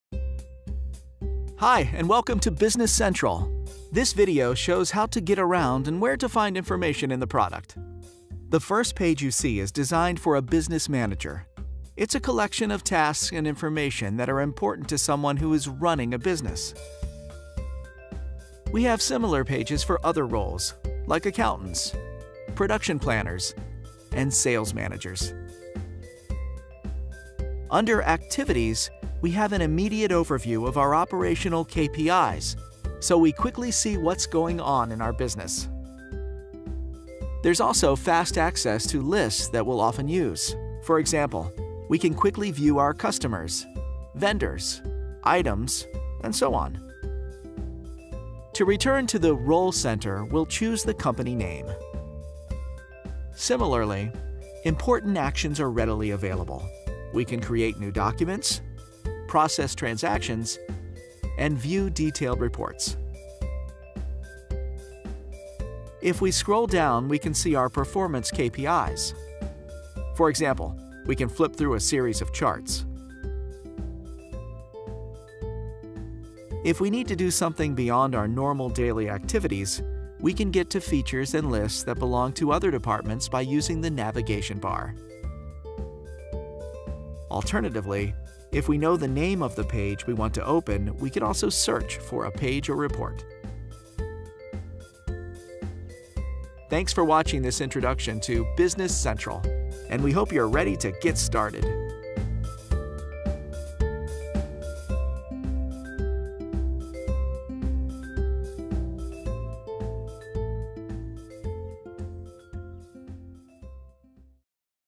Introducción a Microsoft 365 Business Central
Microsoft Dynamics 365 Business Central es una solución conectada con el entorno Microsoft para una experiencia de trabajo más ágil e integrada. Evita duplicidades de datos y mejora la trazabilidad y el análisis del negocio para tomar mejores decisiones.